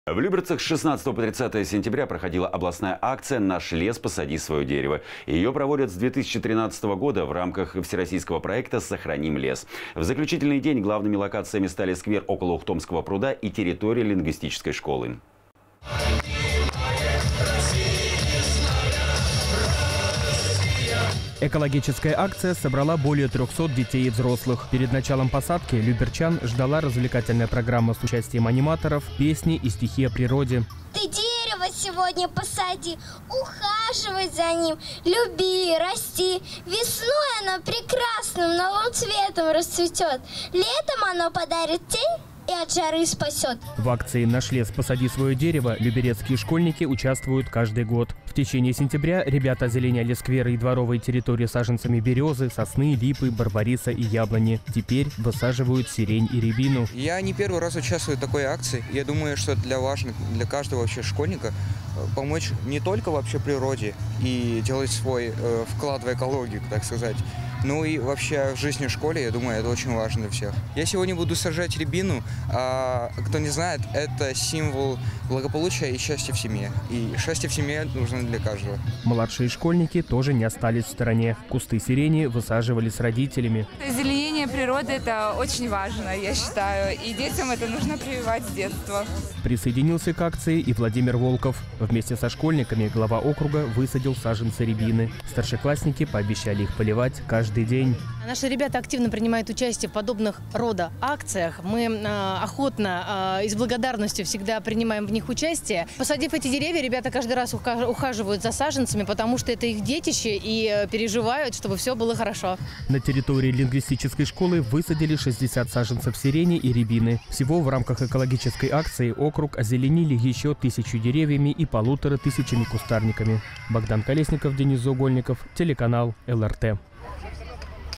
Акция «Наш лес Посади своё дерево» подводит итоги
В Люберцах с 16 по 30 сентября проходила областная акция «Наш лес. Посади своё дерево». Её проводят с 2013 года в рамках всероссийского проекта «Сохраним лес». В заключительный день главными локациями стали сквер около Ухтомского пруда и территория лингвистической школы.

Экологическая акция собрала более 300 детей и взрослых. Перед началом посадки люберчан ждала развлекательная программа с участием аниматоров, песни и стихи о природе.

В акции «Наш лес. Посади своё дерево» люберецкие школьники участвуют каждый год. В течение сентября ребята озеленяли скверы и дворовые территории саженцами берёзы, сосны, липы, барбариса и яблони. Теперь высаживают сирень и рябину.

Давид Карапетян, ученик лингвистической школы г.о. Люберцы: «Я не первый раз участвую в такой акции. Думаю, что важно для каждого школьника помочь не только природе и сделать свой вклад в экологию, так сказать, но и в жизни школы это очень важно для всех. Я сегодня буду сажать рябину. Кто не знает – это символ благополучия и счастья в семье, а счастье в семье нужно для каждого».

Младшие школьники тоже не остались в стороне. Кусты сирени высаживали с родителями.

Присоединился к акции и Владимир Волков. Вместе со школьниками глава округа посадил саженцы рябины. Старшеклассники пообещали их поливать каждый день.

Наталья Соловьева, директор лингвистической школы г.о. Люберцы: «Наши ребята активно участвуют в подобного рода акциях. Мы охотно и с благодарностью всегда принимаем в них участие. Посадив эти деревья, ребята каждый раз ухаживают за саженцами, потому что это их детище, и переживают, чтобы всё было хорошо».

На территории лингвистической школы высадили 60 саженцев сирени и рябины. Всего в рамках экологической акции округ озеленили ещё тысячью деревьями и полутора тысячами кустарниками.